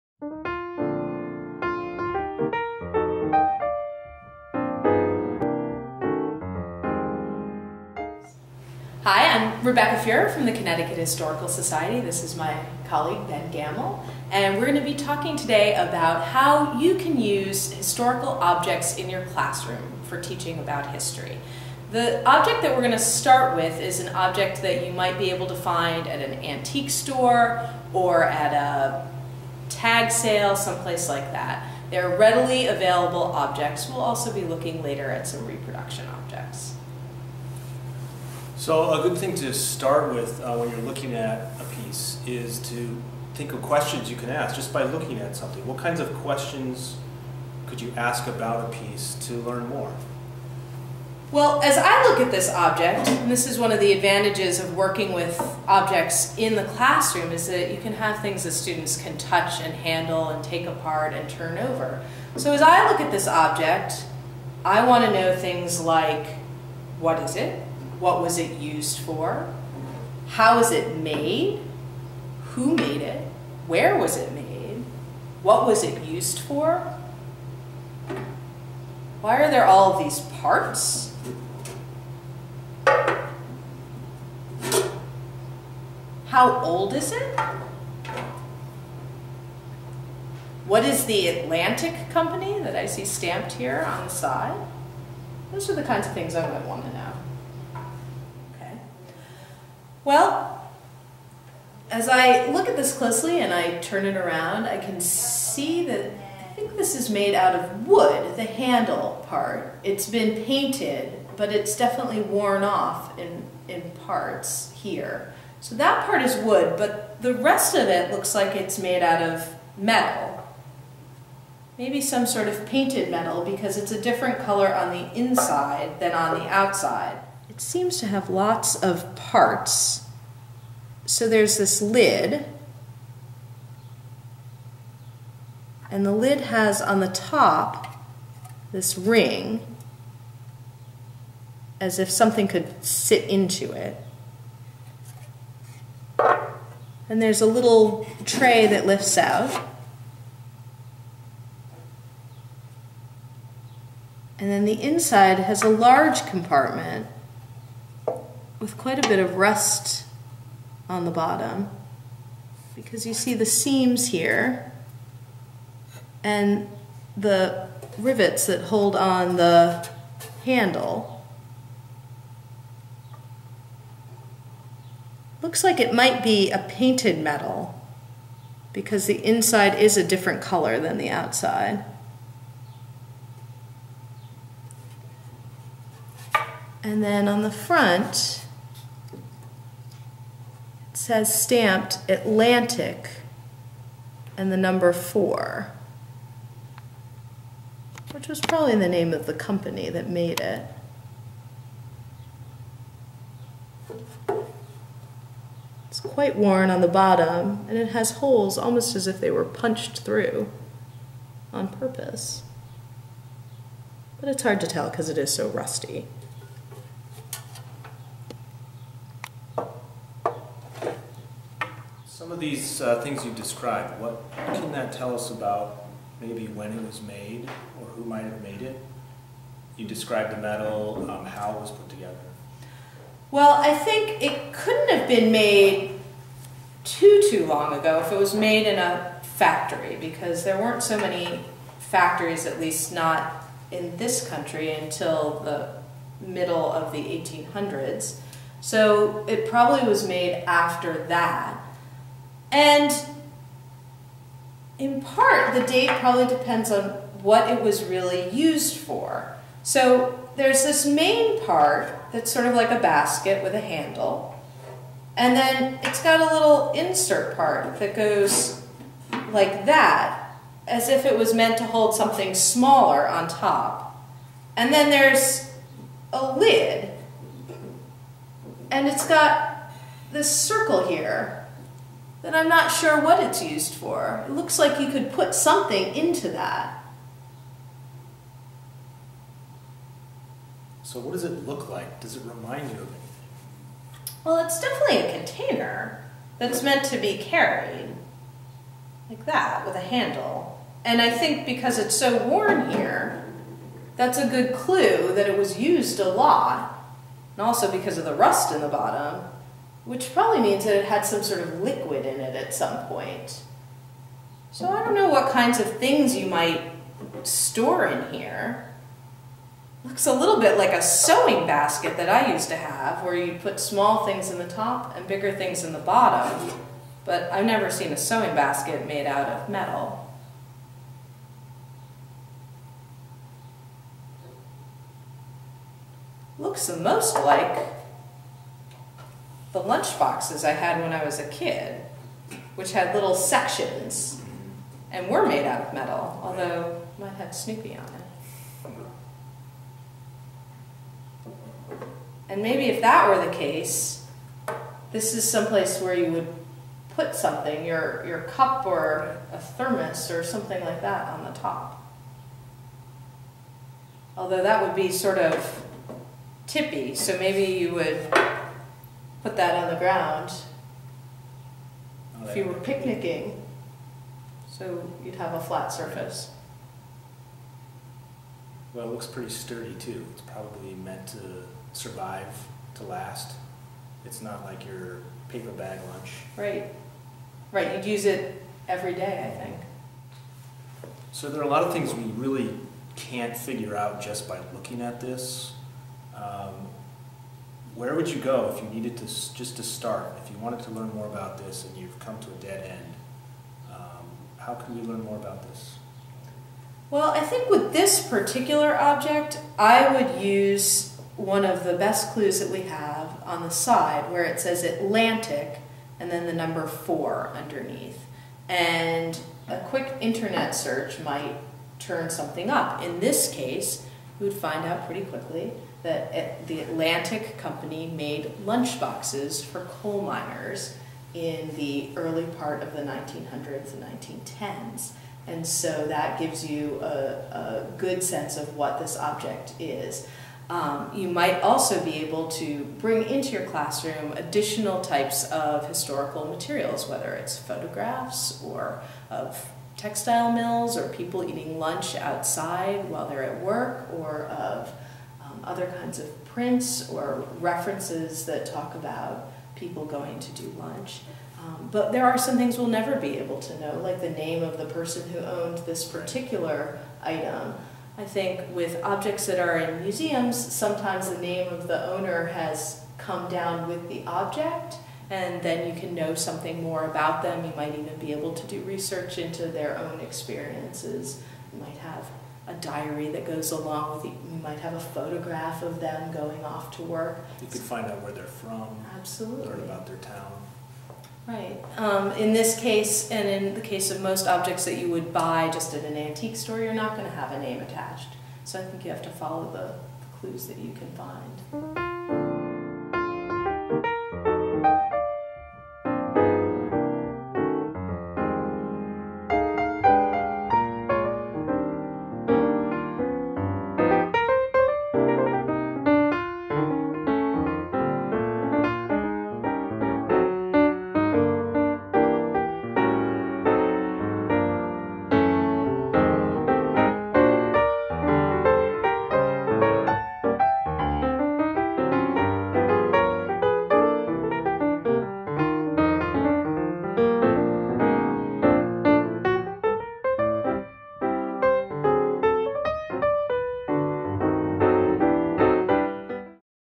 Object Identification at the Connecticut Historical Society
The process of how to identify historical objects is described at the Connecticut Historical Society. This is a model for how classroom teachers can bring history into their classroom.